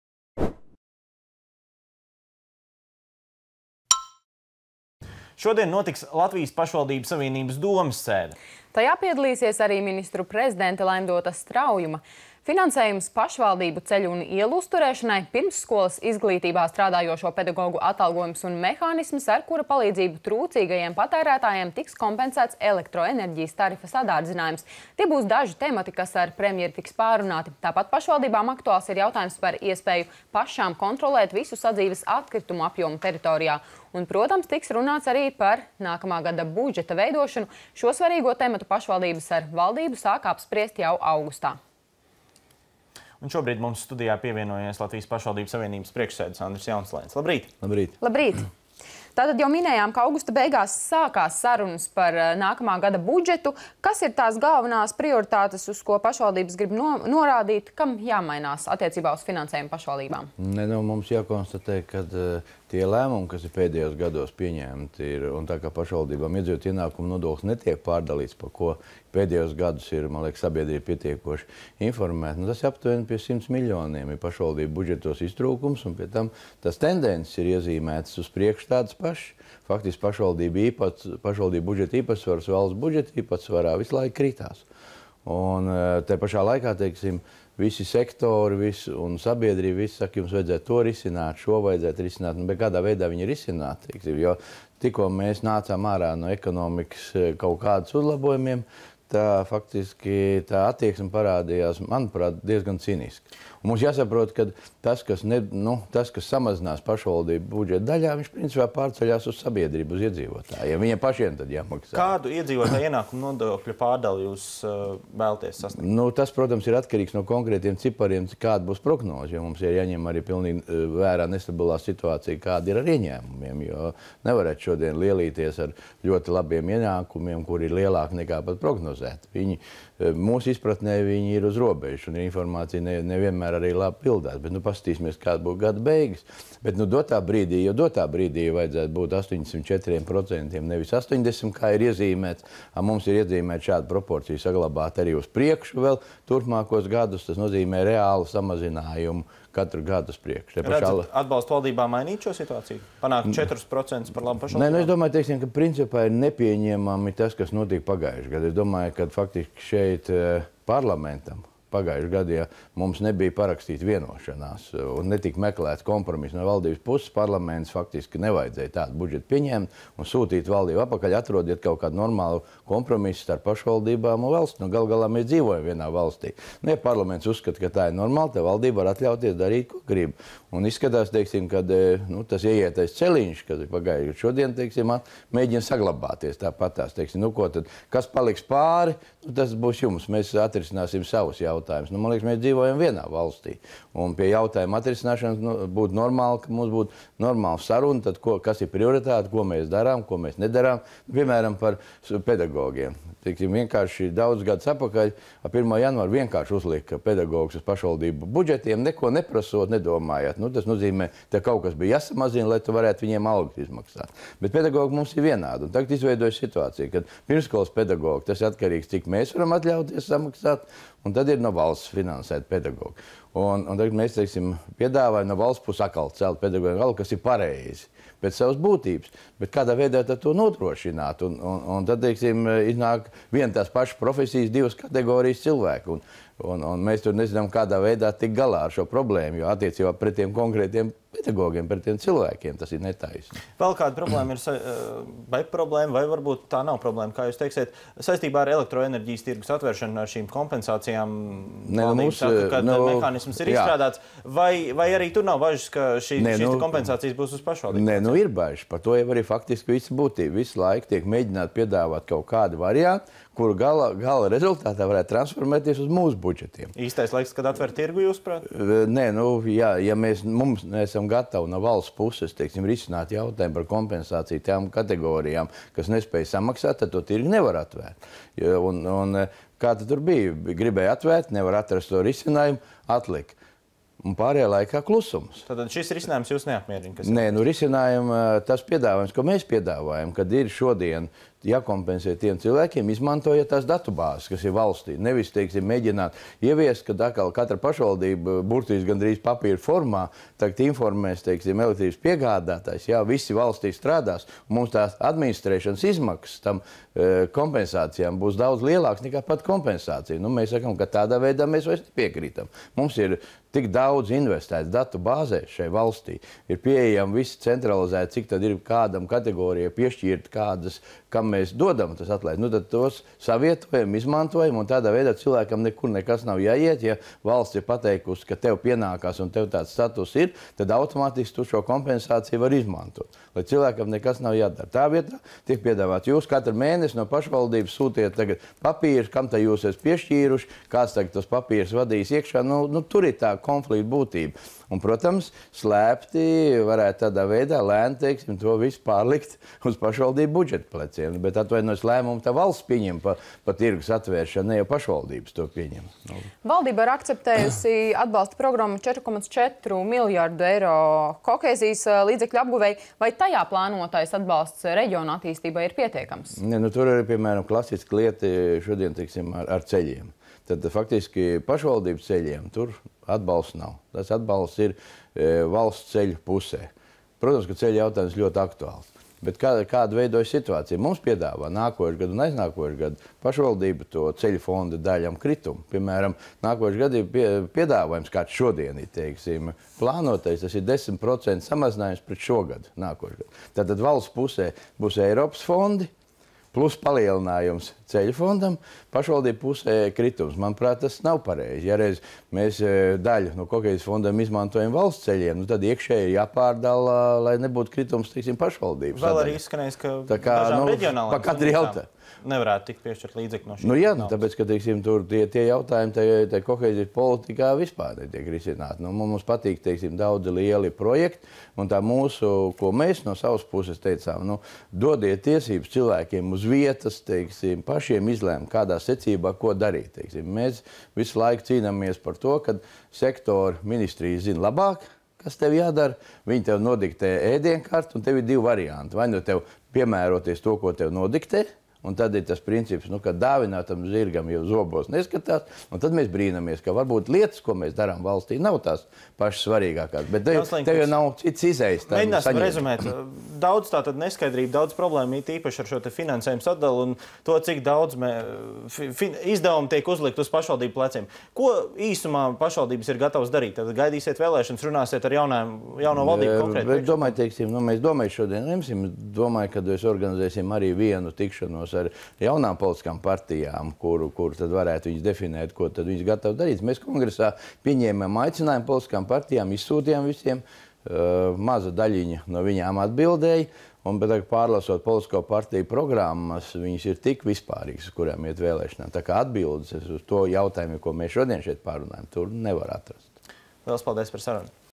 Intervija ar Andri Jaunsleini
Šodien notiks Latvijas Pašvaldību savienības domes sēde.
Tajā piedalīsies arī Ministru prezidente Laimdota Straujuma.
Finansējums pašvaldību ceļu un ielu uzturēšanai, pirmsskolas izglītībā strādājošo pedagogu atalgojums un mehānisms, ar kura palīdzību trūcīgajiem patērētājiem tiks kompensēts elektroenerģijas tarifa sadārdzinājums – tie būs galvenie temati, kas ar premjeri tiks pārrunāti. Tāpat pašvaldībām aktuāls ir jautājums par iespēju pašām kontrolēt visu sadzīves atkritumu apjomu teritorijā. Un, protams, tiks runāts arī par nākamā gada budžeta veidošanu. Šo svarīgo tematu pašvaldības ar valdību sāka apspriest jau augustā.
Šobrīd mums studijā pievienojies Latvijas pašvaldību savienības priekšsēdis Andris Jaunsleinis.